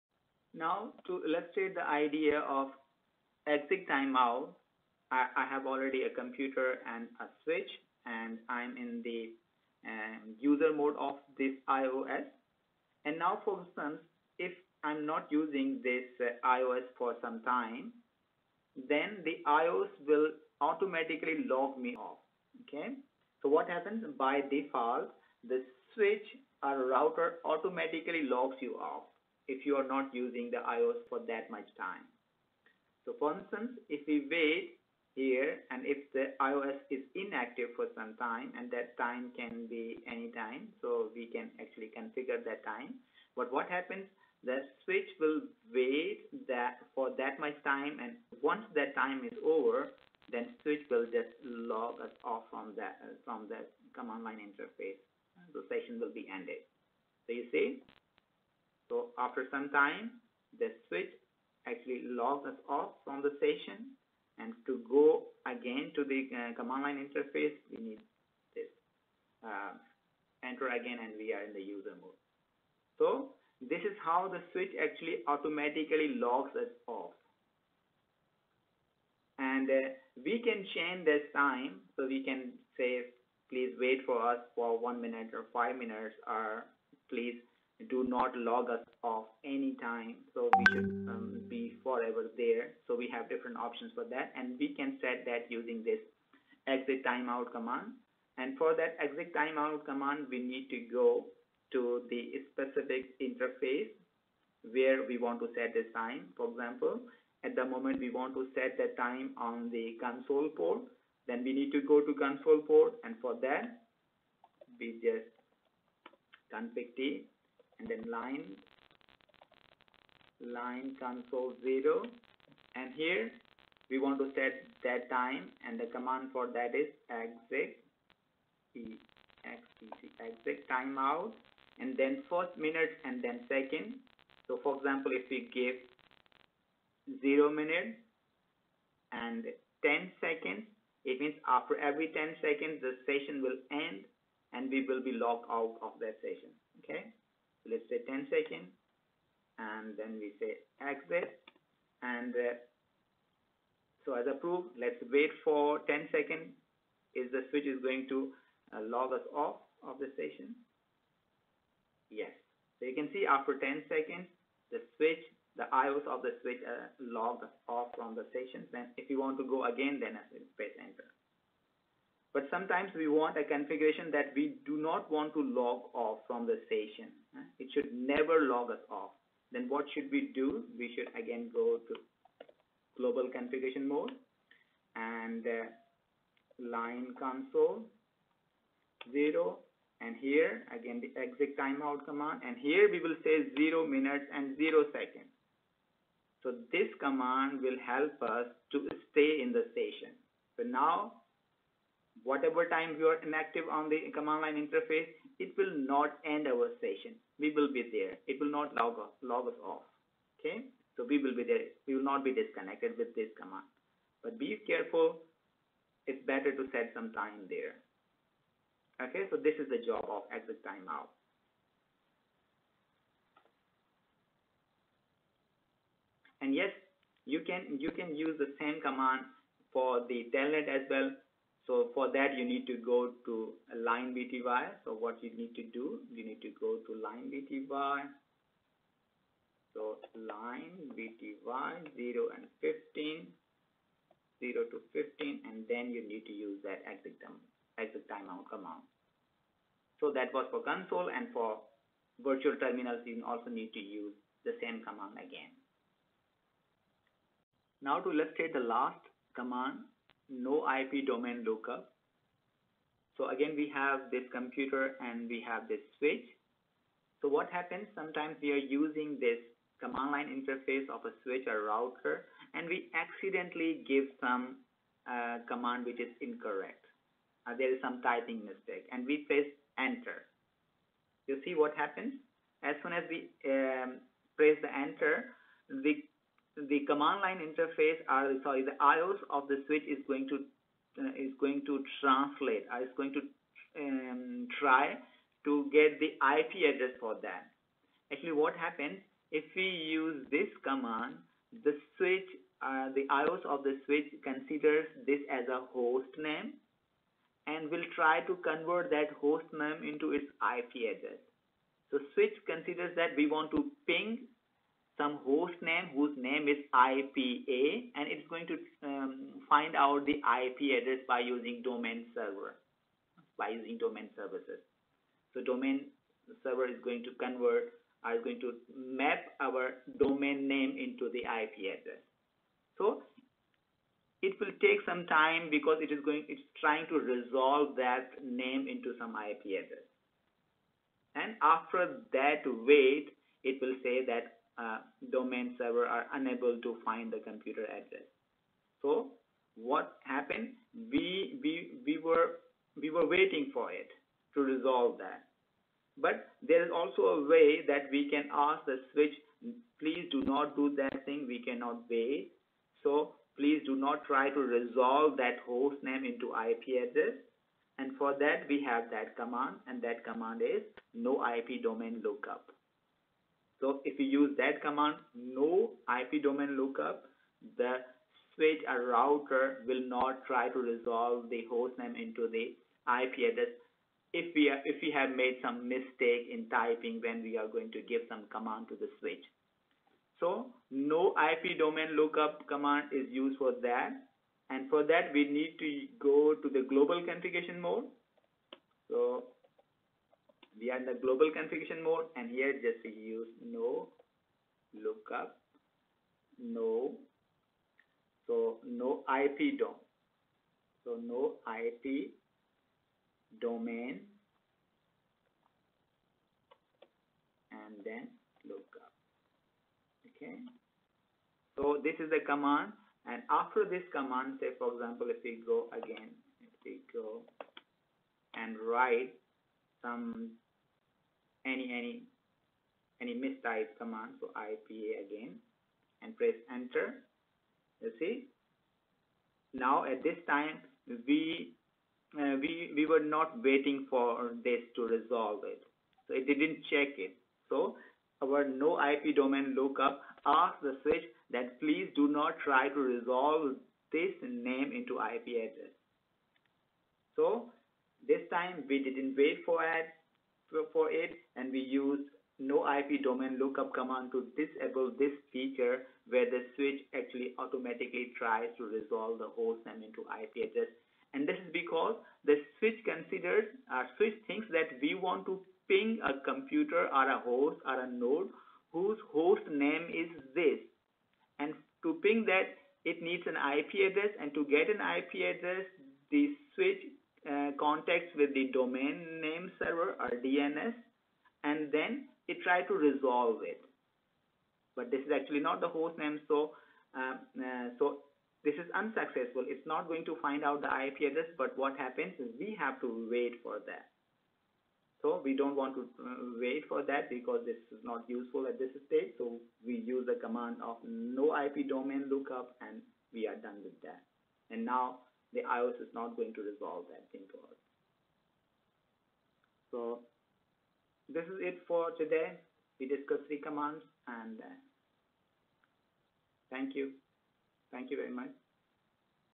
exec timeout | no ip domain-lookup | syslog in Packet Tracer
4:20 no ip domain-lookup
During our use of CLI of Cisco IOS, sometime we get disturbed by the syslog messages.
Syslog messages are the information about the events, errors or any serious problems happening in the switch/ router.
We can avoid those messages.
No logging synchronous is used to hold the notification until there is any input from keyboard or no output

The command used for that is
(config) line console 0
(config-line) logging synchronous

To disable the syslog messages, use the following command
(config) no logging console

To disable DNS resolution by switch, use
(Config) no ip domain lookup

To set the time for Cisco switch or router to log-off the current session for certain amount of time, use the following command
(config) exec-timeout min second